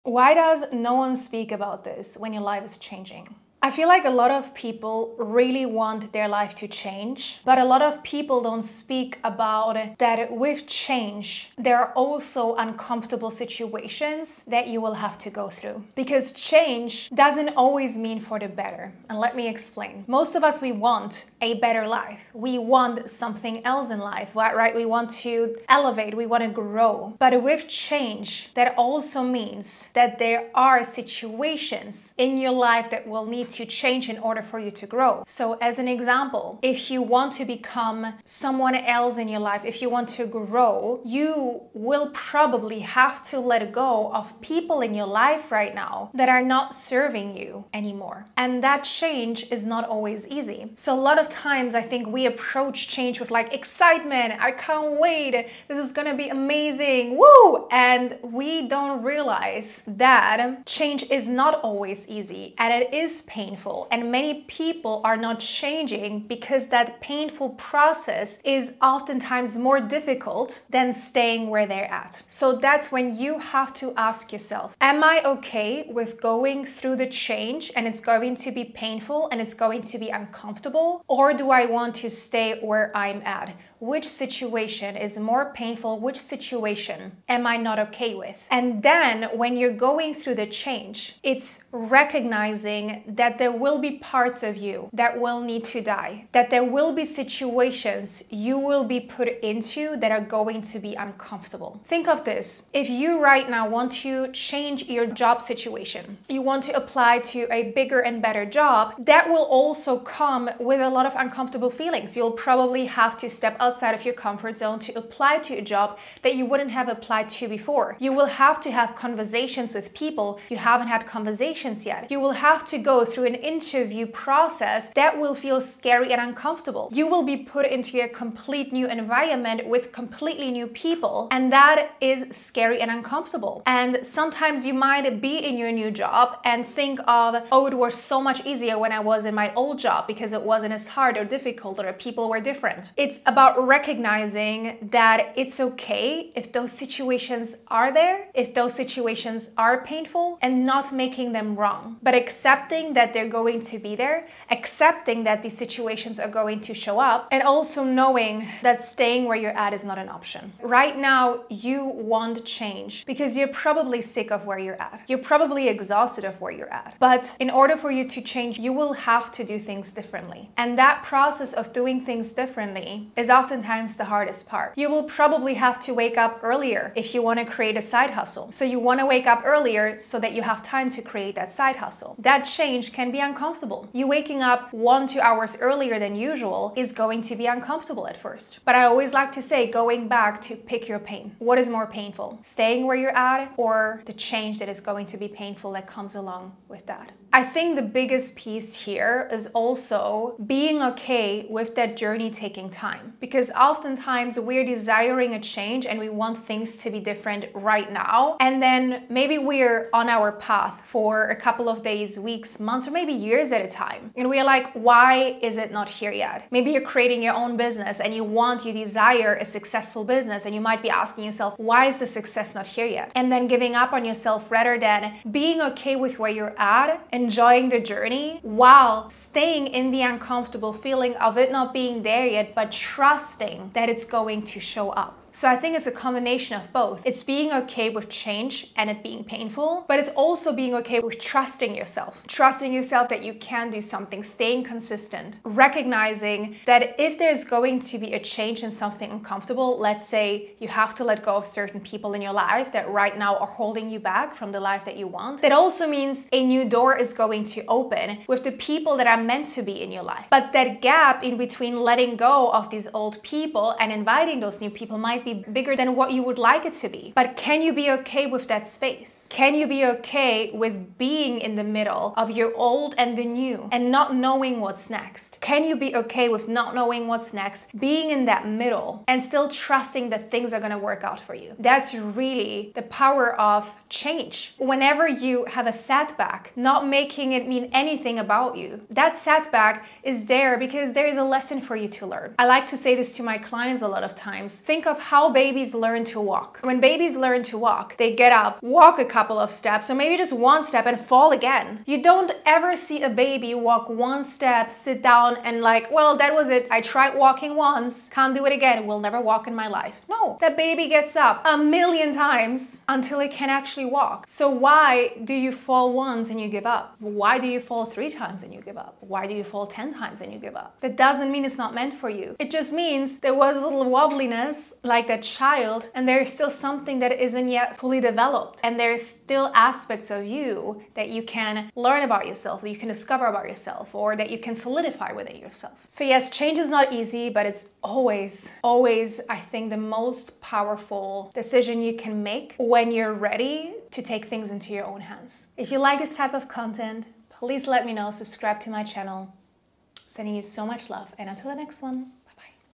Watch this if you have a hard time changing your life
Change can be painful at times, but it is also about recognizing that it is okay if it is and choosing the pain of staying where you are at or changing your life.